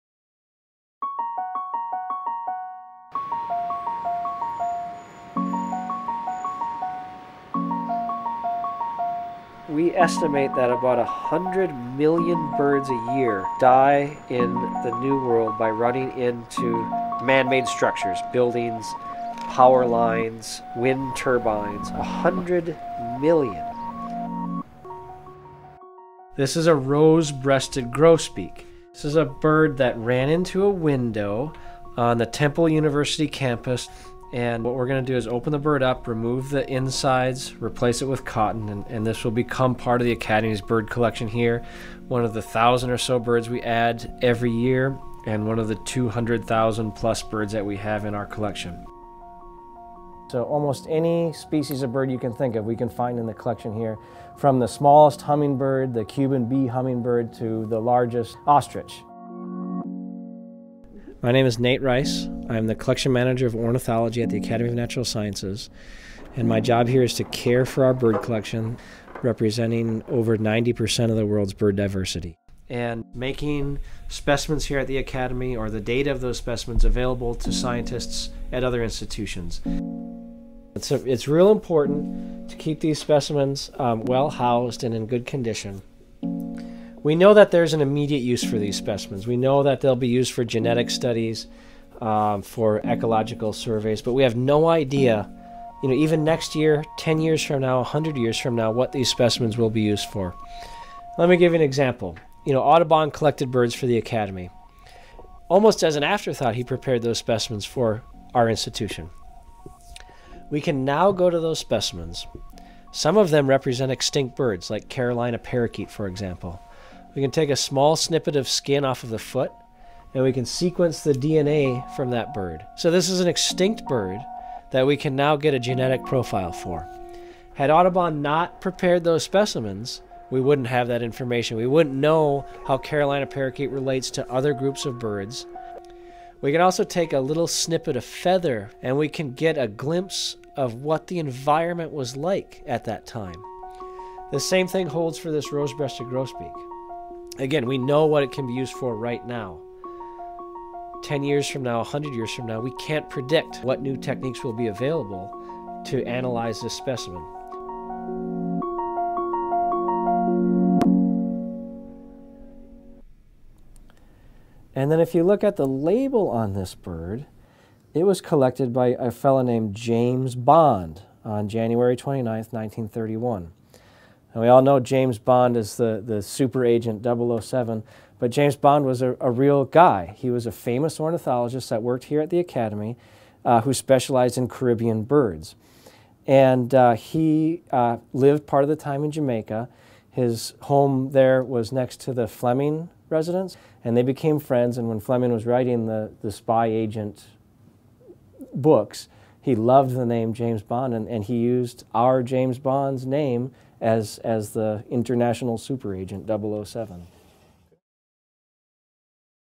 The Ornithologist
Only a fraction of the collection at the country's oldest natural history museum is out in the open for visitors to see.
Eighteen million plant and animal specimens are catalogued and stored in huge metal cases for research purposes upstairs at the Academy of Natural Sciences of Drexel University.
Nate Rice, collection manager of ornithology, spends most of his time caring for and managing the more than 200,000 bird specimens there.